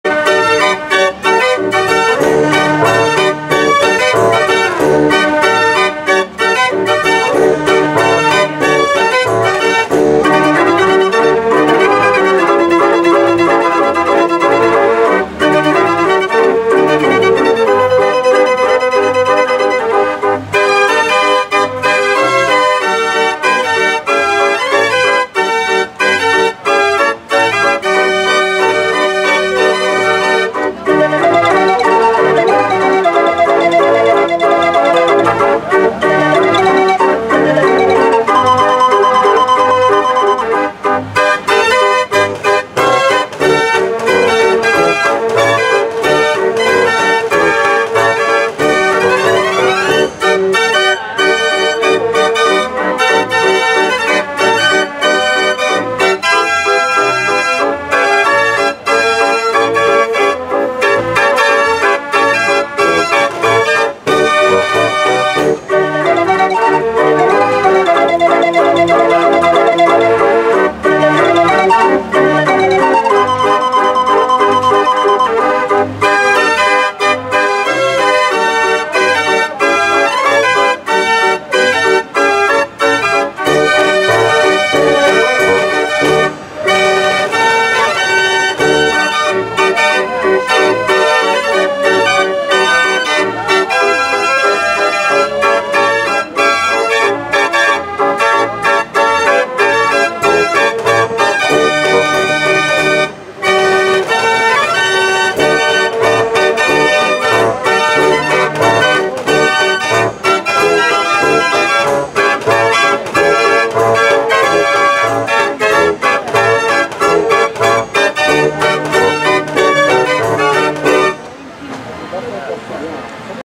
KDV Leek #16 12-09-2021
13 Draaiorgels, de meeste opgenomen.
35er Ruth, De Paardekop, De Aventura, De Westfries, De Hartenvrouw, De Grote Dorus,
Wellershaus, De Lange Gavioli, De Grote Cap, De Lekkerkerker, Steck Pianola
Veel plezier, Wim & Greta